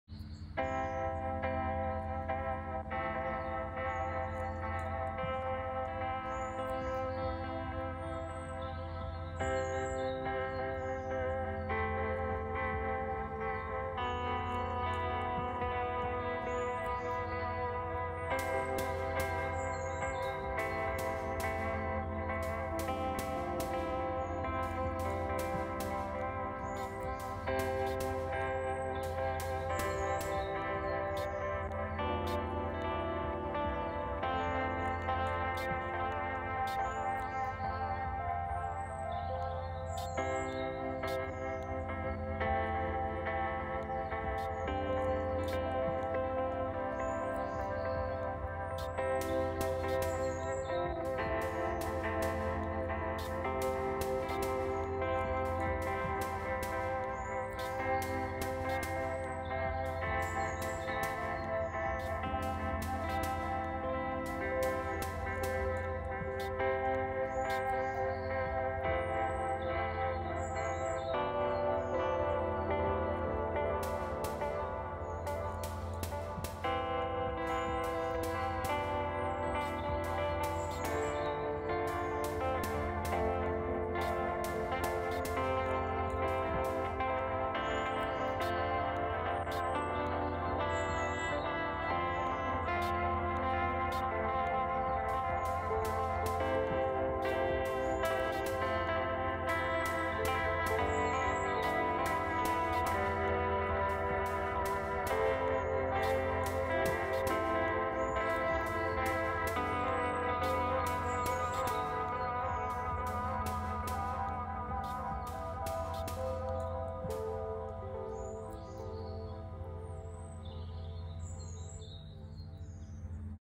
Boughs
Playing around with some modulated analogue and tape delays.

Clip is of a water vole I spotted on a walk